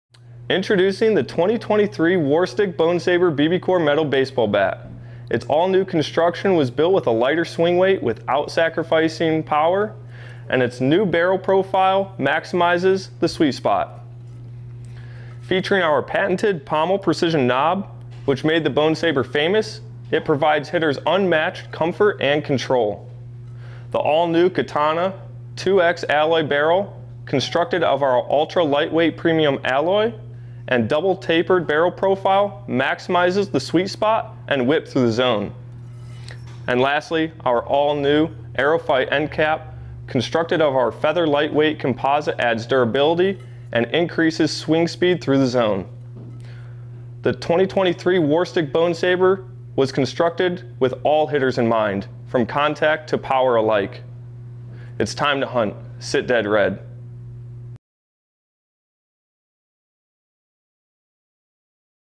The All New Bonesaber BBCOR Metal Bat
Learn more about the fully redesigned Bonesaber BBCOR Metal Bat.

The all new Bonesaber is built to face the most feared pitchers in the game. The Katana1X Alloy Barrel is constructed of our highest grade ultra light alloy and features an extended barrel that increases swing speed and whip thru the zone, improves feel and maximizes sweet spot.

The new Bonesaber is more balanced. Capable of producing superior swing speed and incredible exit velocity. The combination of our patented Pommel Precision Knob paired with one piece construction will give you unmatched comfort and bat control for the most responsive bat in the game.